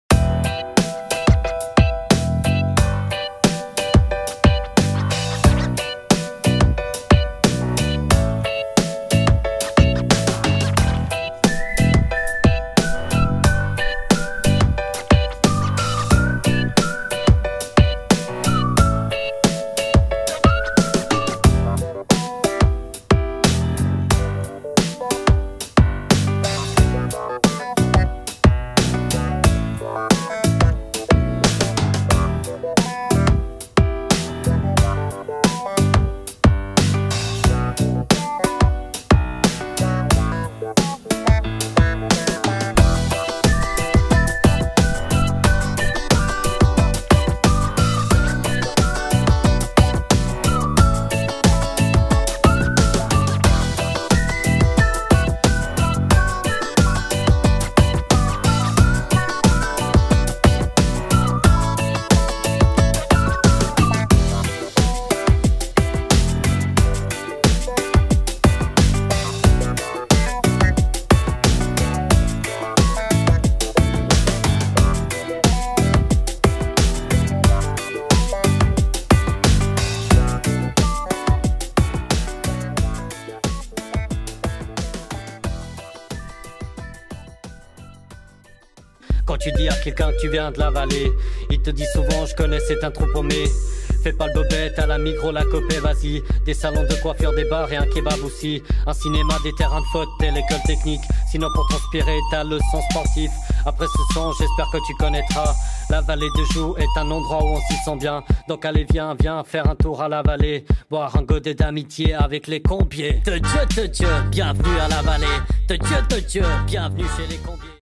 Le Zap vaudois (3-8 octobre)
24heures.ch vous propose un tour d'horizon des vidéos postées sur youtube par des internautes vaudois durant la semaine écoulée.